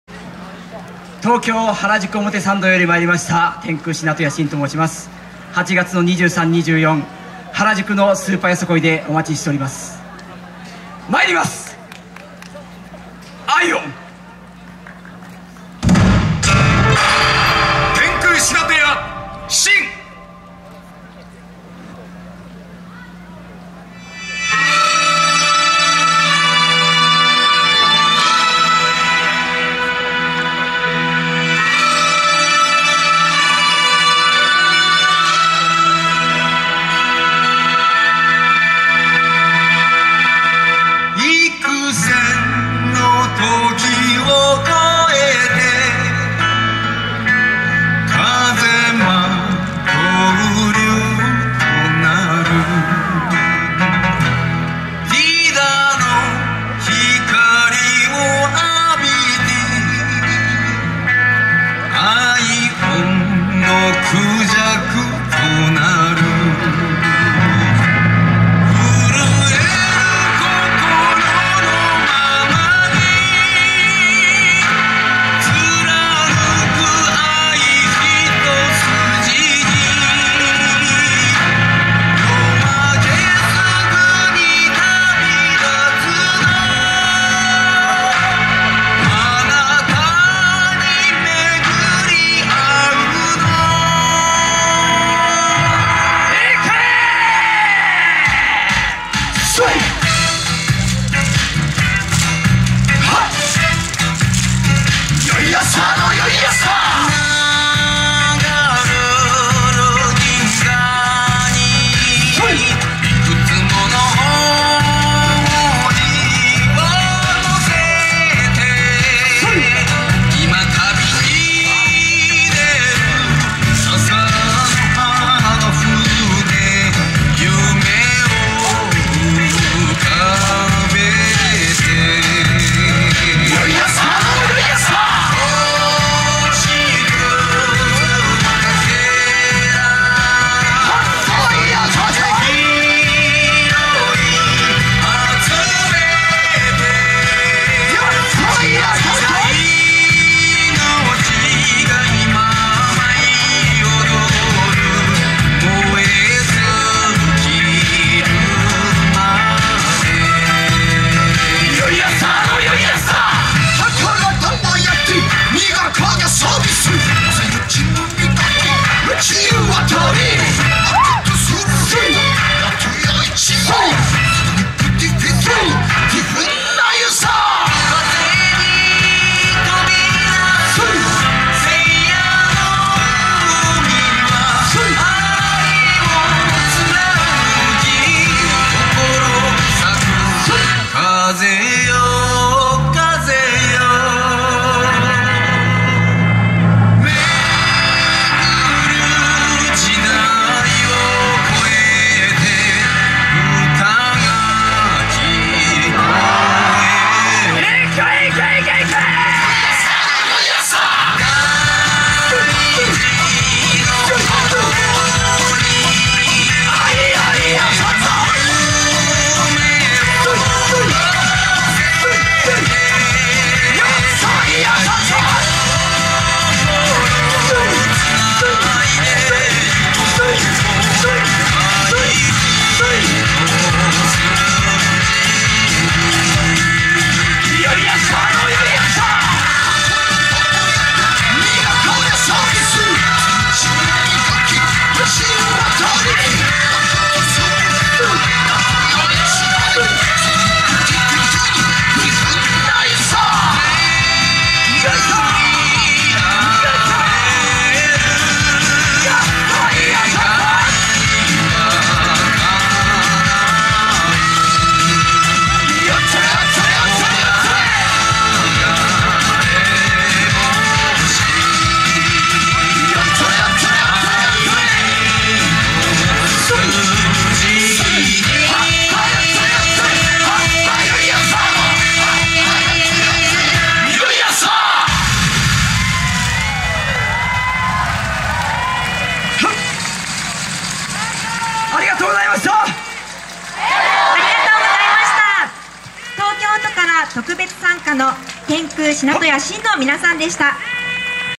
第１６回よさこいとやま 天空しなと屋しんさん
2014/08/03富山市で開催された、第１６回よさこいとやま　城址公園特設ステージでの、天空しなと屋しんさんの演舞です。